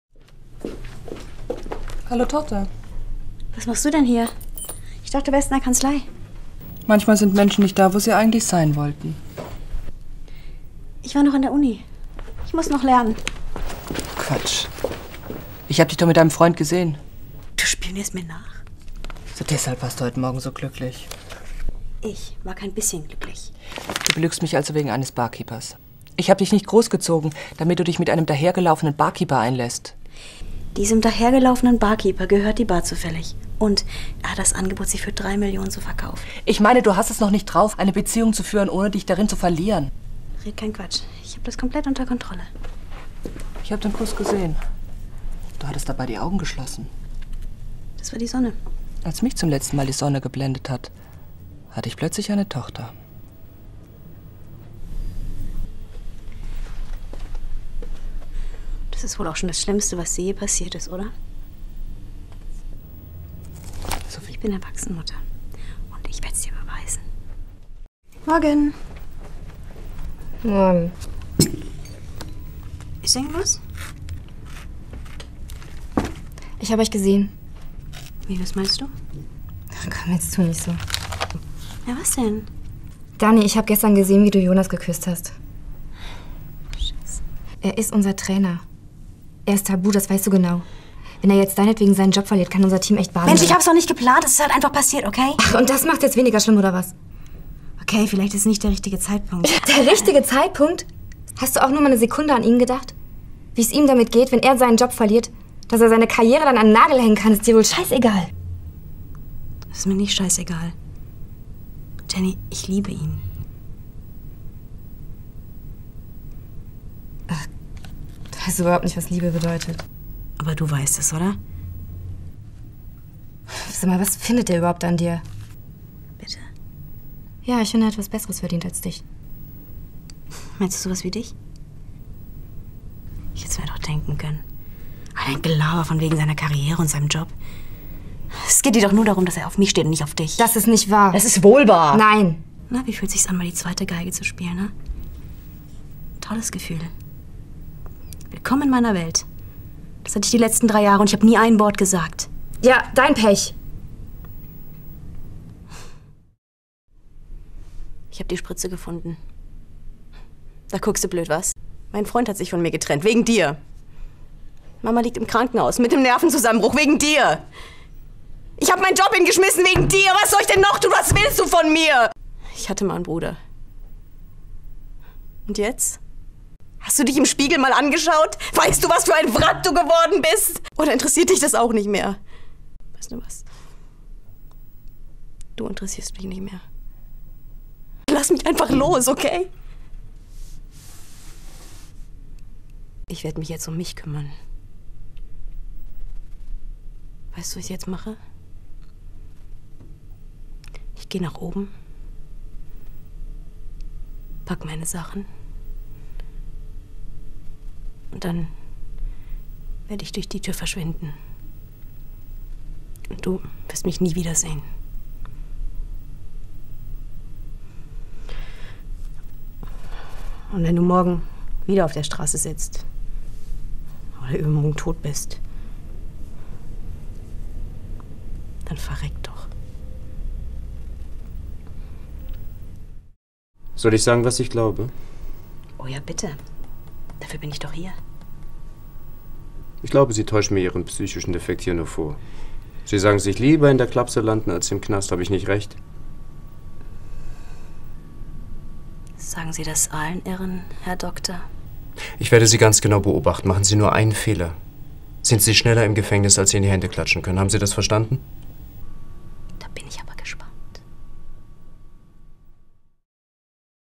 Bettina Kenney - Showreel 01/2010
Bettina Kenney
Szenen:
1 - "Geblendet", 0:00 - 1:08
2 - "Slam Dunk" 1:09 - 2:36
3 - "Mein Drogenbruder" - Montage 2:37 - 3:53
4 - "Irrenhaus" 3:54 - 4:24

Regie: Detlef Rönfeldt
Schnitt: Marco Giese / Actors Workshop Hamburg

Entstanden im Rahmen des Camera Actors Studio des iSFF. Leitung: Detlef Rönfeldt.